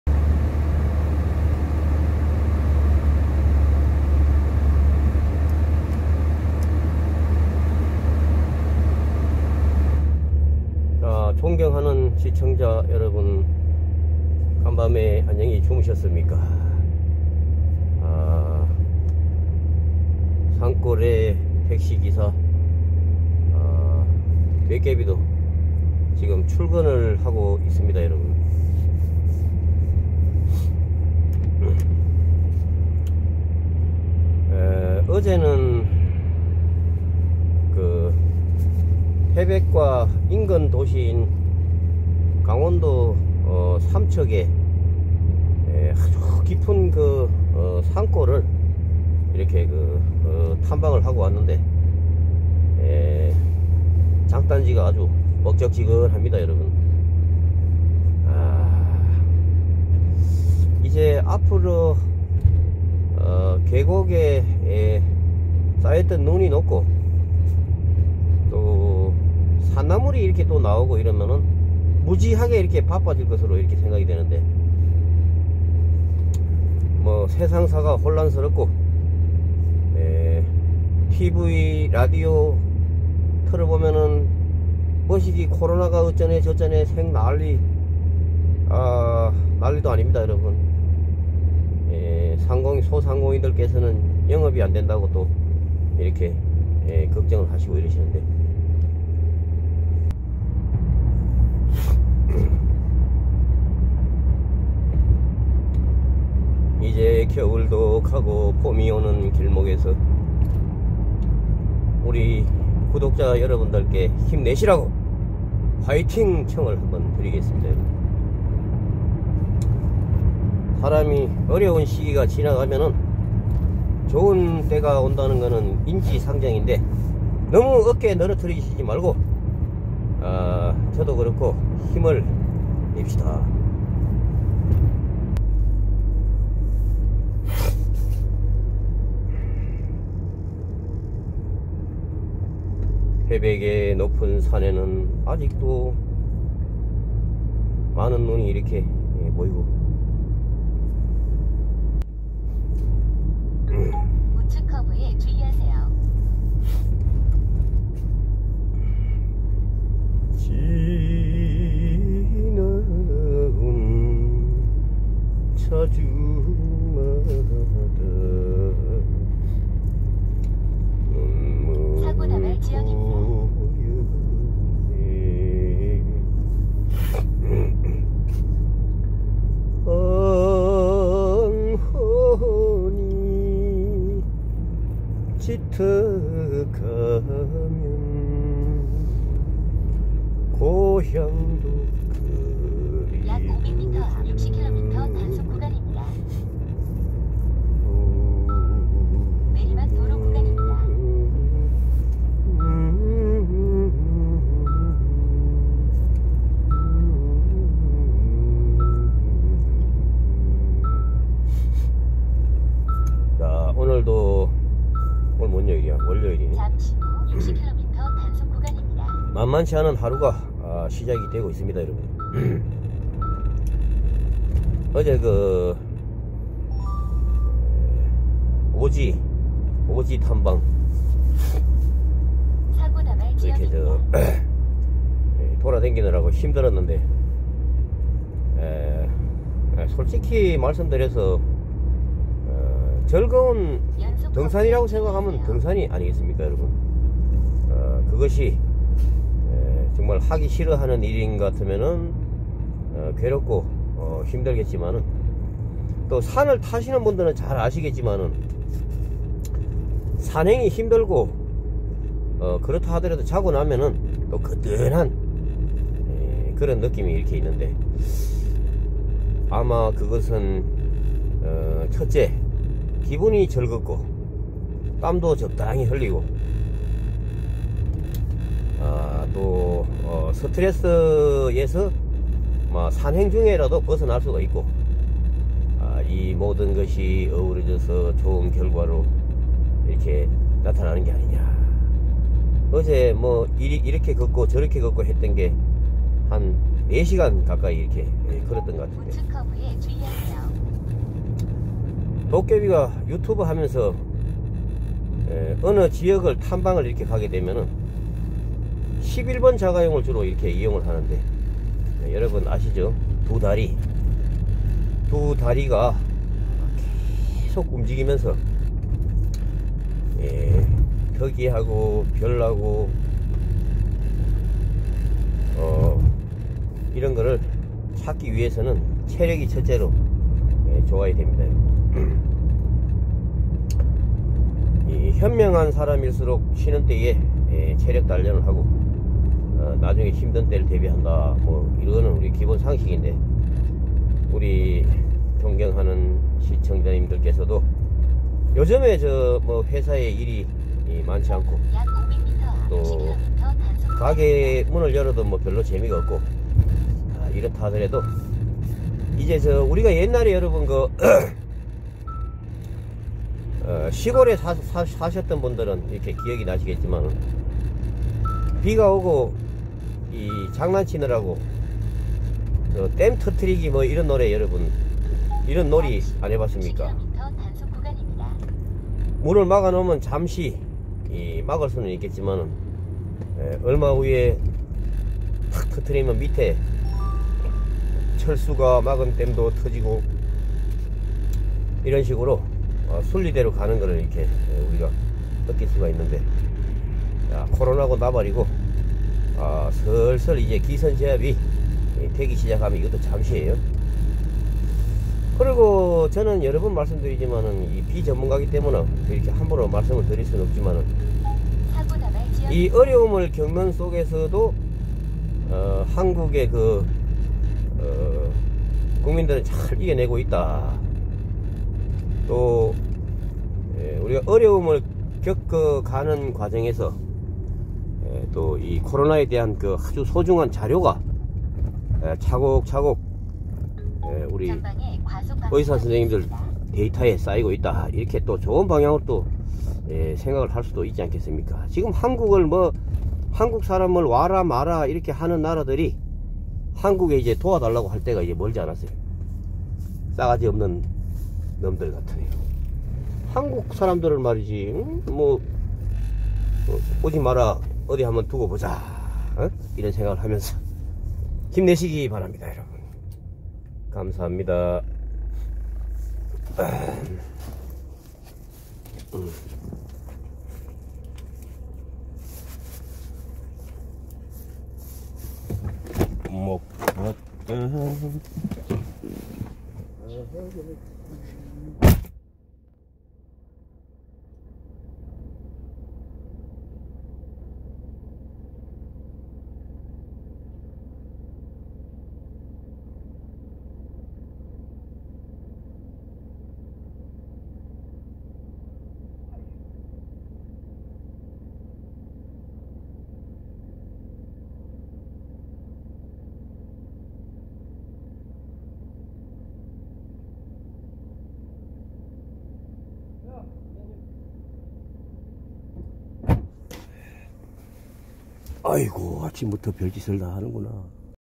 산골의 도깨비 택시기사 즐건맘 출근중 어제 오지탐방 불구 거뜬함 태백 삼척시
산골의 도깨비 택시 기사는 오늘도 기분좋게
일터로 출근중 웃으며 삽시다 힘냅시다 중얼